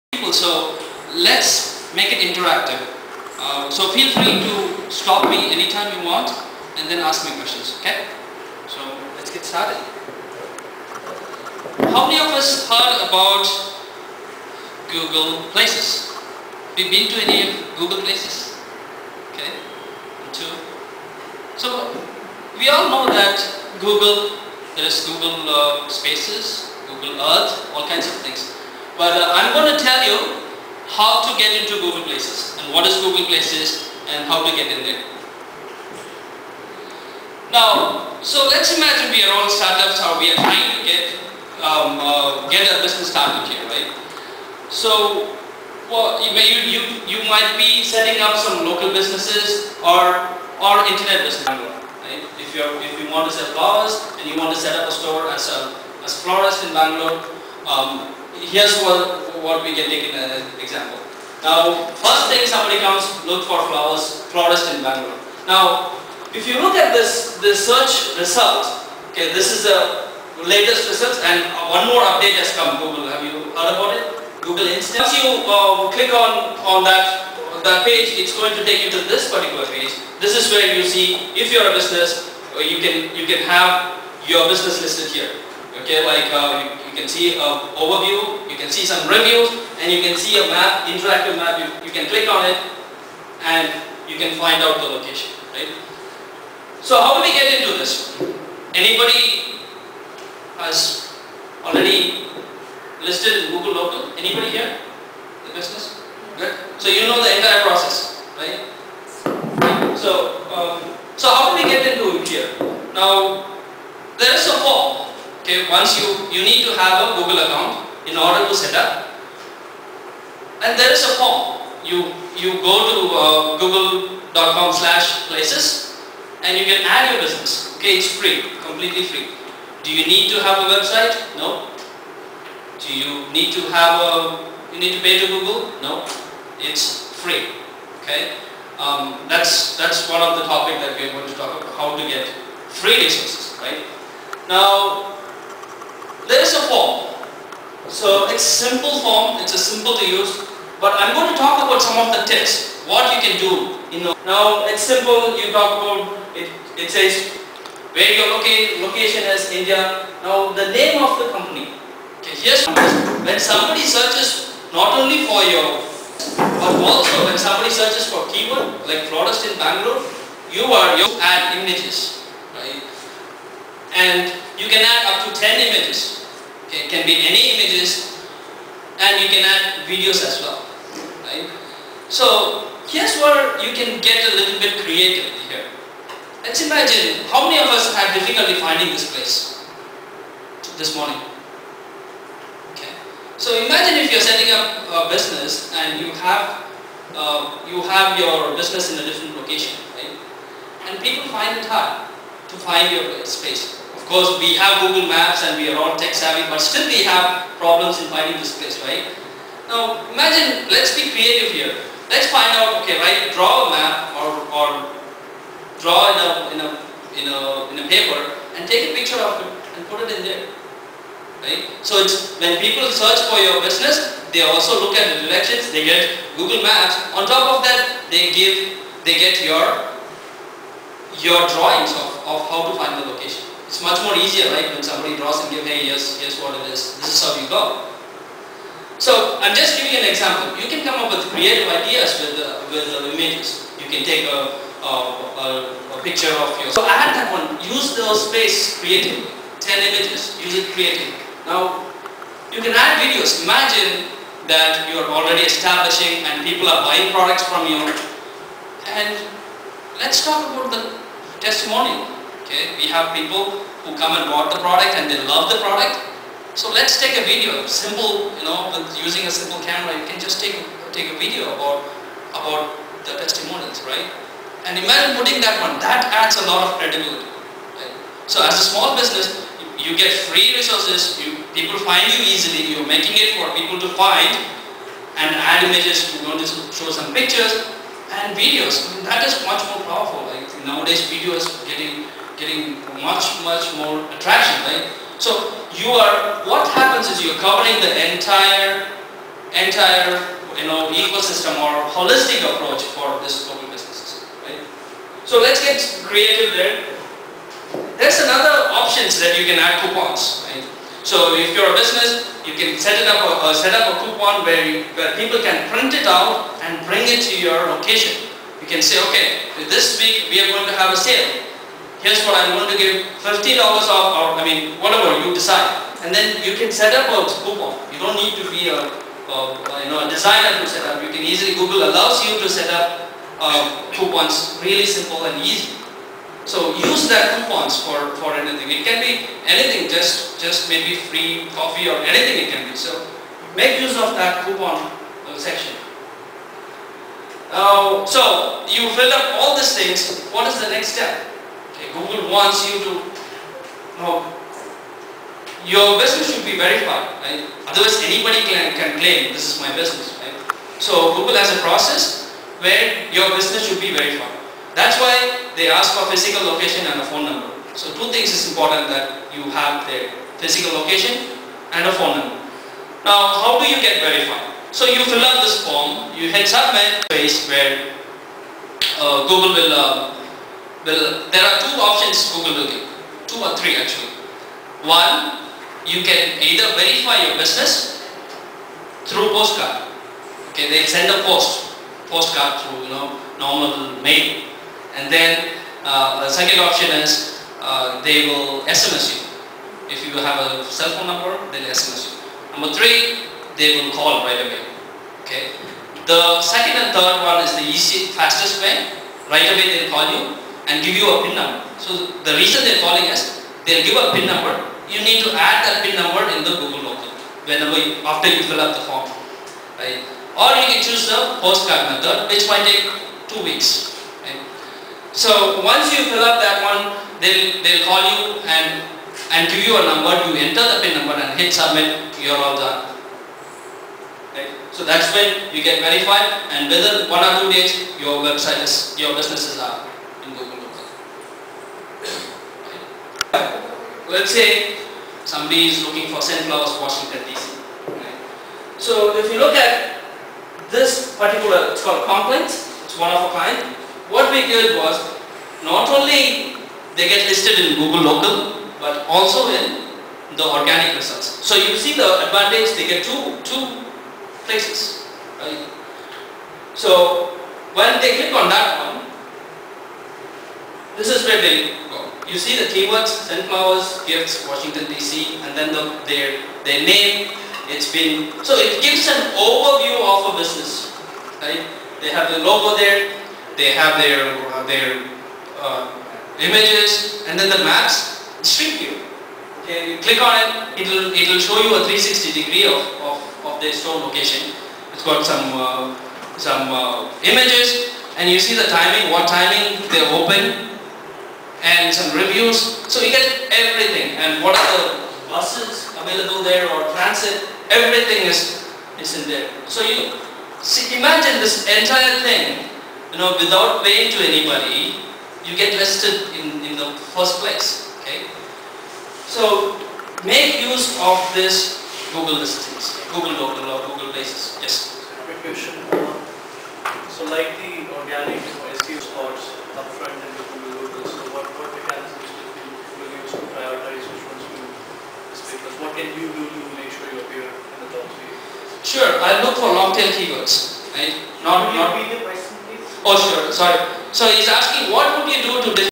SEO Speakers Bangalore. Videos Startup Saturday Bangalore How to Get Listed in Google Places- Part 1
SEO Videos, Start up Saturday , Head Start Bangalore,  Suresh SEO Bangalore, Consultant in House SEO expert Bangalore. Speaking about - How to Get Listed in Google Places- Part 1